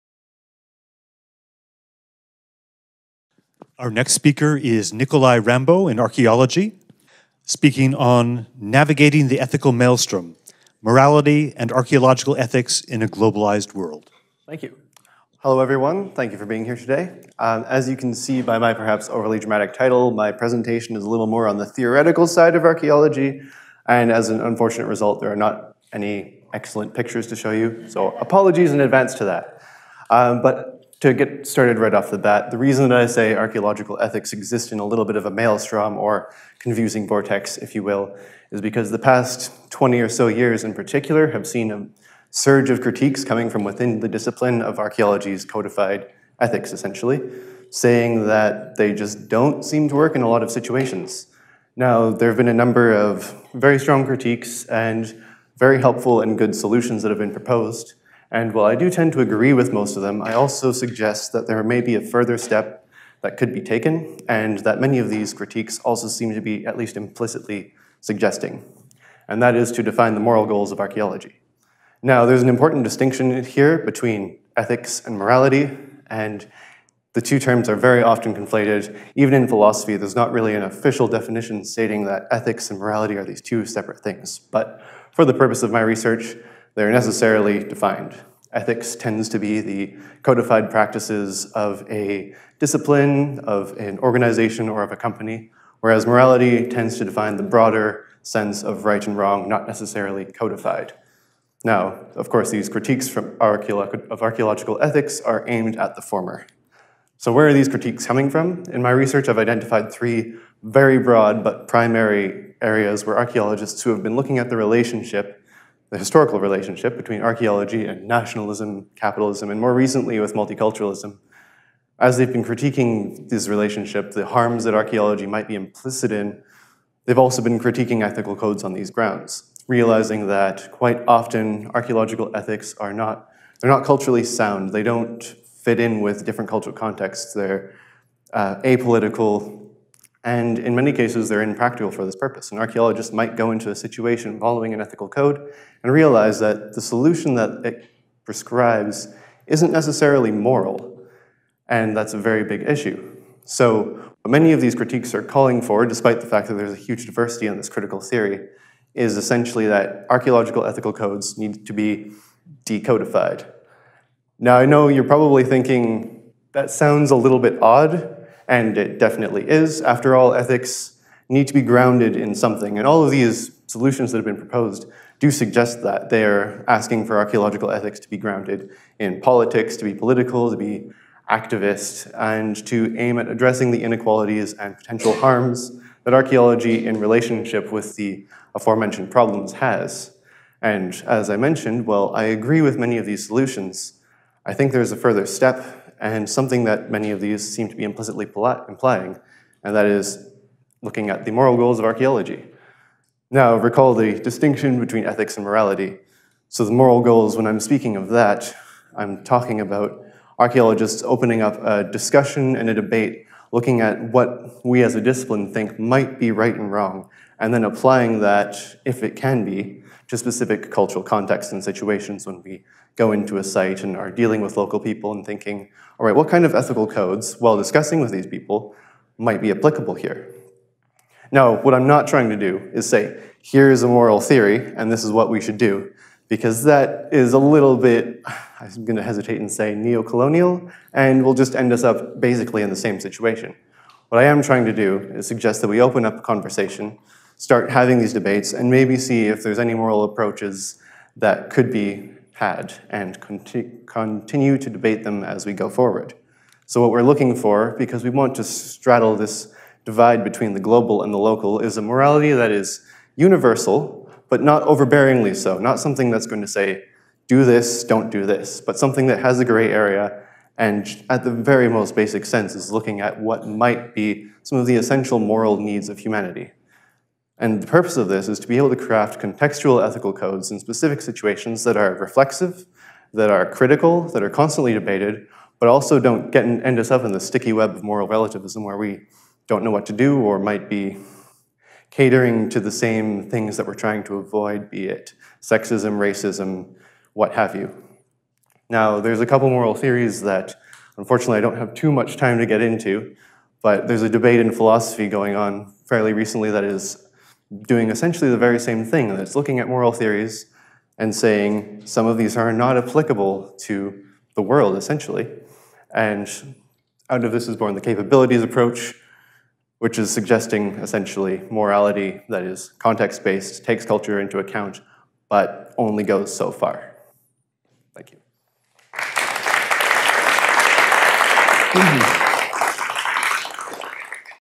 Nikolai Rambo | Undergraduate Student Symposium 2018
Nikolai Rambo presents his Three Minute Thesis "Navigating the Ethical Maelstrom: Morality and Archaeological Ethics in a Globalized World" at The Presentation Studio at SFU's Big Data Hub | March 13, 2018

Interested in hosting a similar presentation, workshop or discussion at The Presentation Studio?